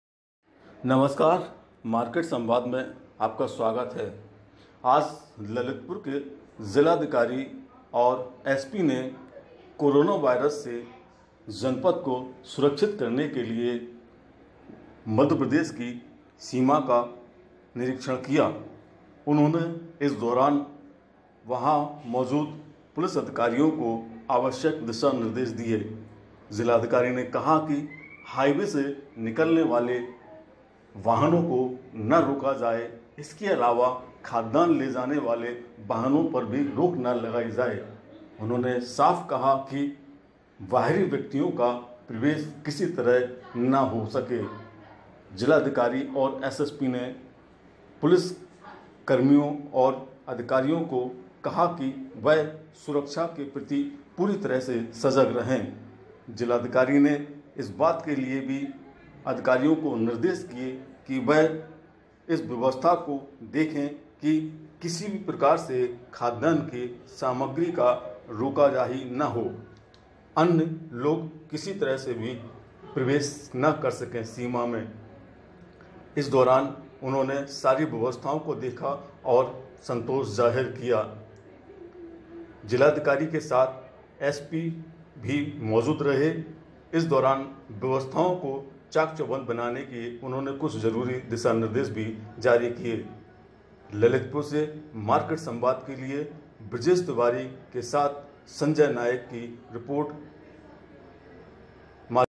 ललितपुर-यूपी-मप्र बॉर्डर देखने क्यों पहुँचे डीएम-एसपी?,रिपोर्ट-बृजेश तिवारी
कोरोनावायरस के बचाव करने के लिए आज जनपद के जिला अधिकारी योगेश कुमार शुक्ल व पुलिस अधीक्षक कैप्टन एमएम बेग ने ललितपुर तालबेहट झांसी मार्ग पर माताटीला मैं मध्य प्रदेश से उत्तर प्रदेश में प्रवेश करने माताटीला बॉर्डर को चेक किया वह सारी दिशाएं सील करा दी गई सील कोट की चेकिंग करने के लिए आज उपरोक्त दोनों अधिकारी कड़कड़ाती धूप में जांच करते हुए और ड्यूटी पर लगे पुलिसकर्मियों को सख्त निर्देश दिए कि किसी भी प्रकार से कोई भी व्यक्ति मध्य प्रदेश से यूपी में नआ सके